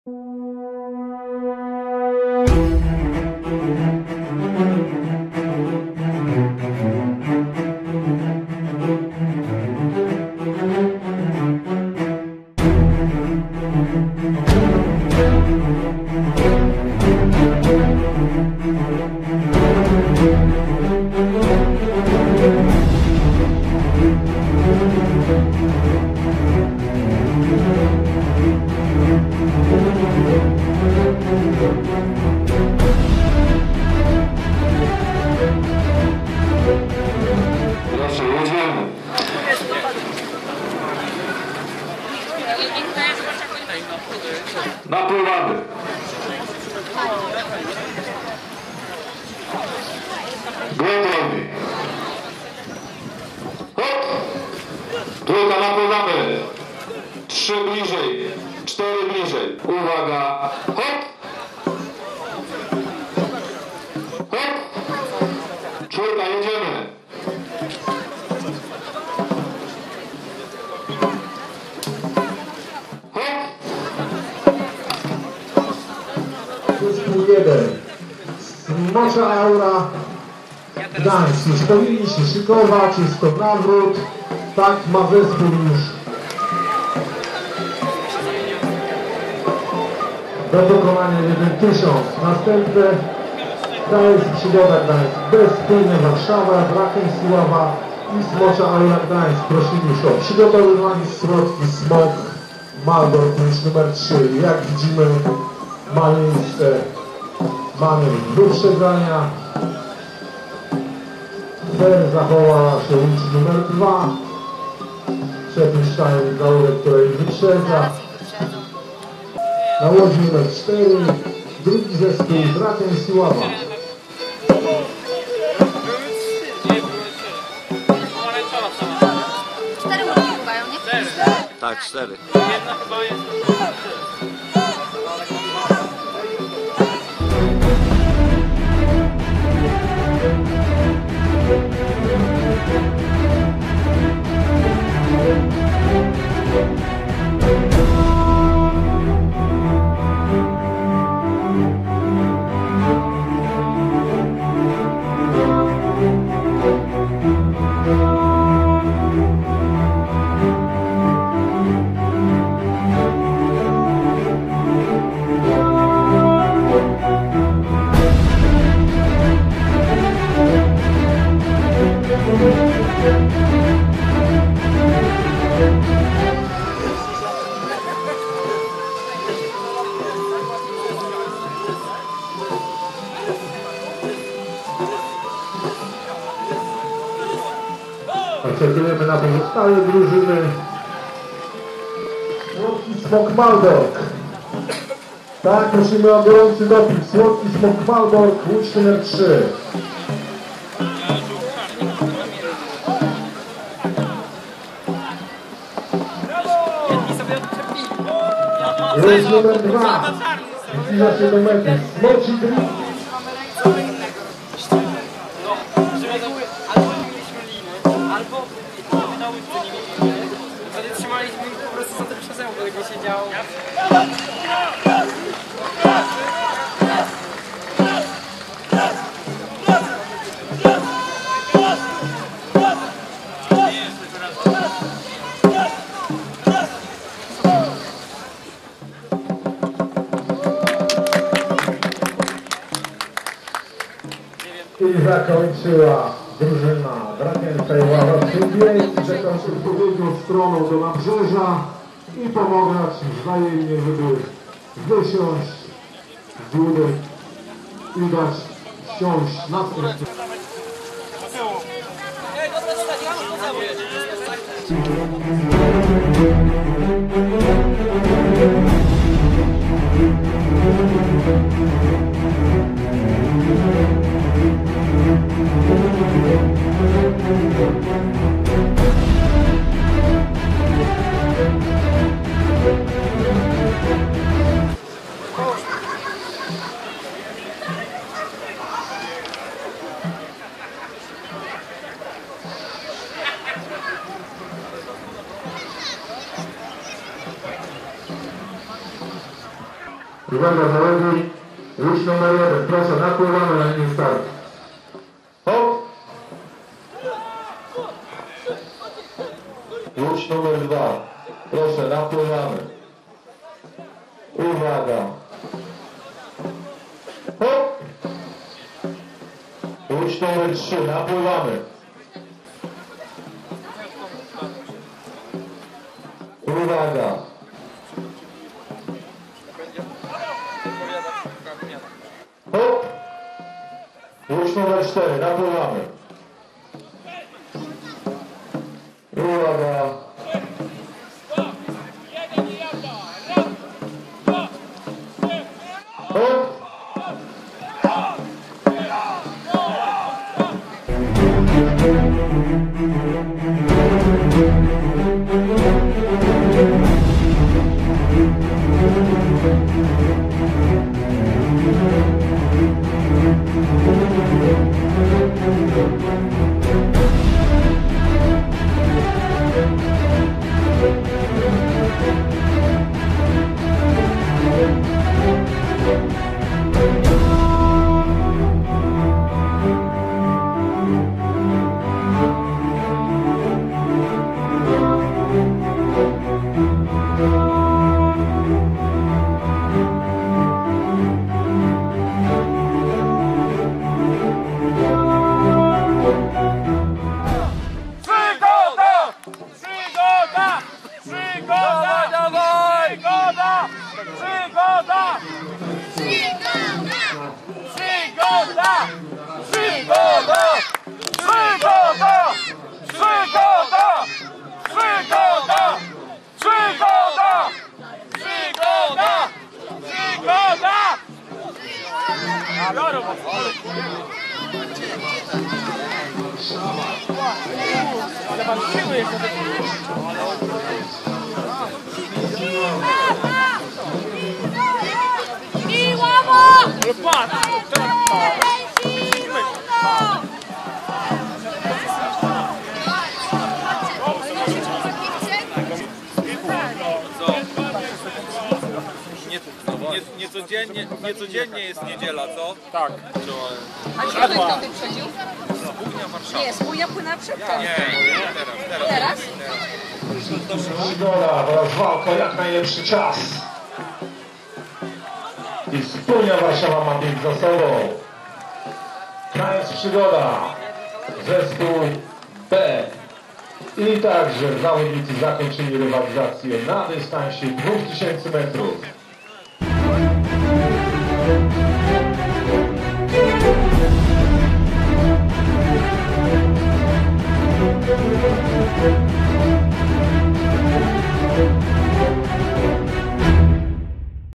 Oblężenie Malborka. Regaty Smoczych Łodzi – 2019, cz 3.
Wyścig na 2000 m.